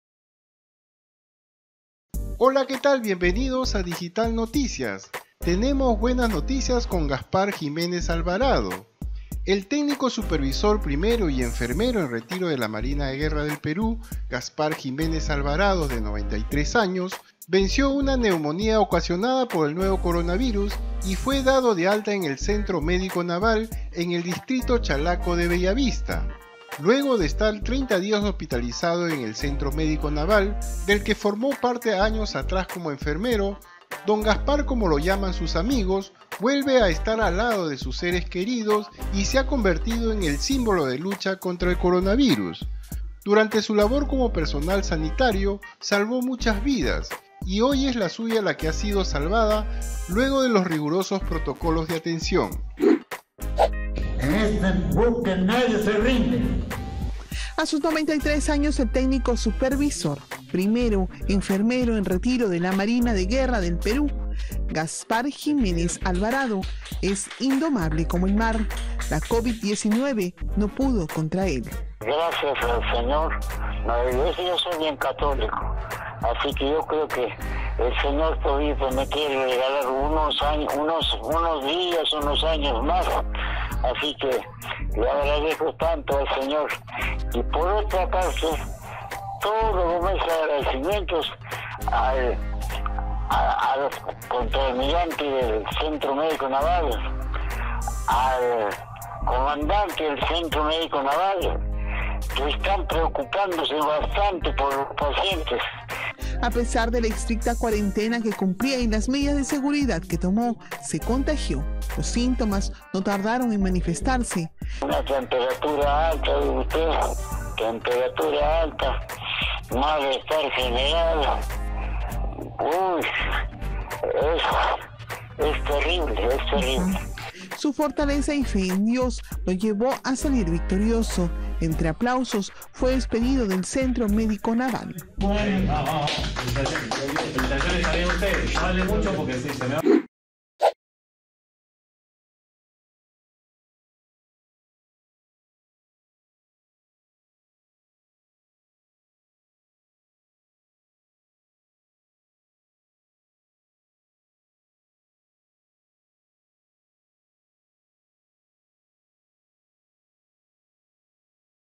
ANCIANO DE 93 AÑOS VENCIÓ AL COVlD-19 EN EL PERÚ
El técnico supervisor primero y enfermero en retiro de la Marina de Guerra del Perú Gaspar Jiménez Alvarado, de 93 años, venció una neumonía ocasionada por el nuevo coronavirus y fue dado de alta del Centro Médico Naval, en el distrito chalaco de Bellavist
FUENTES :
CANAL N: